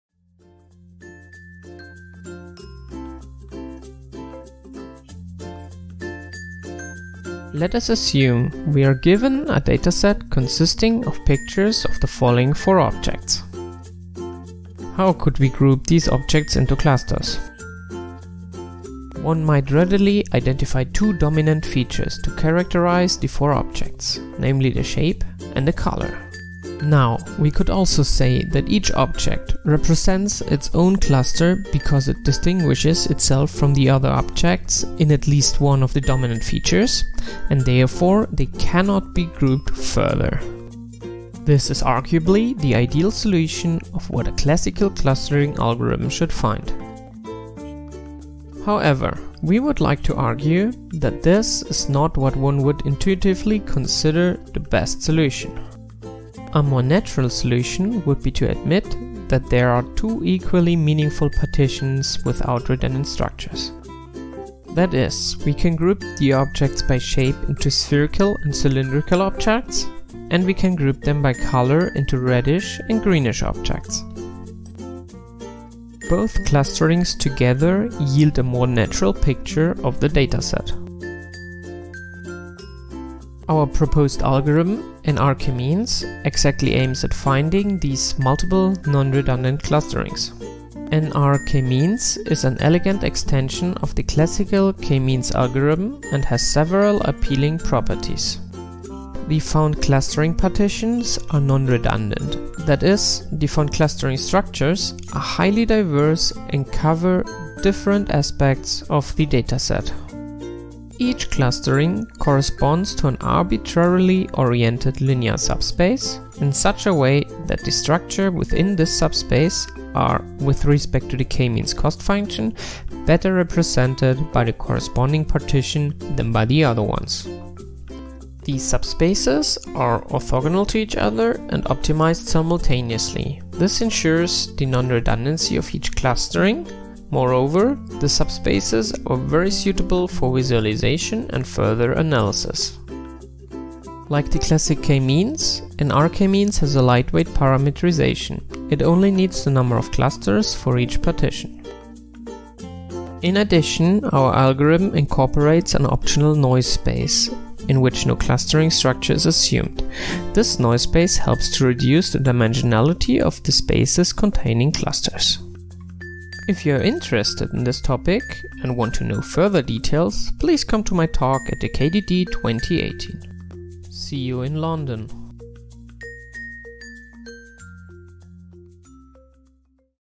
Discovering Non-Redundant K-means Clusterings in Optimal Subspaces
Authors:
Dominik Mautz (Ludwig Maximilian University of Munich); Wei Ye (Ludwig Maximilian University of Munich); Claudia Plant (University of Vienna); Christian Böhm (Ludwig Maximilian University of Munich)